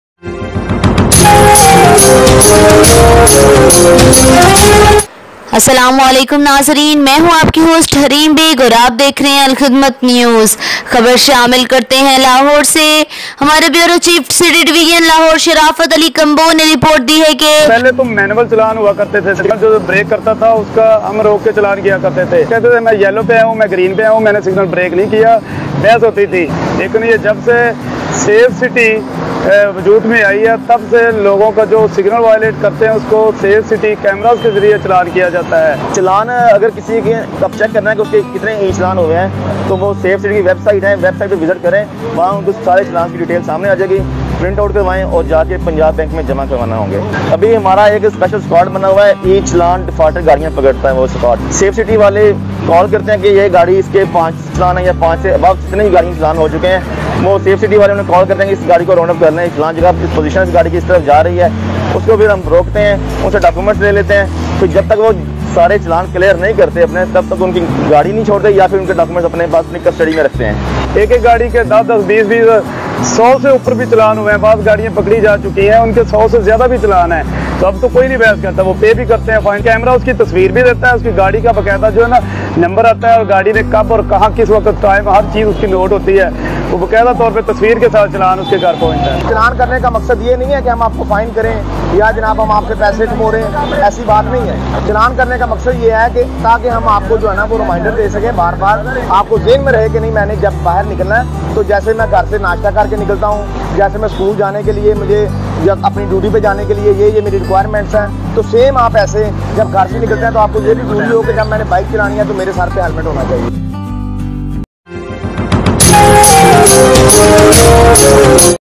AK NEWS | CTPL ۔Your life is very precious
آپکی زندگی بہت قیمتی ھے!!
ذرا سی غفلت آپکے لیے نقصان کا باعث بن سکتی ھے!!
قانون پر عمل کریں اور مہذب شہری ھونے کا ثبوت دیں!!
آپکی معاون اور مددگار!!!
سٹی ٹریفک پولیس لاھور!!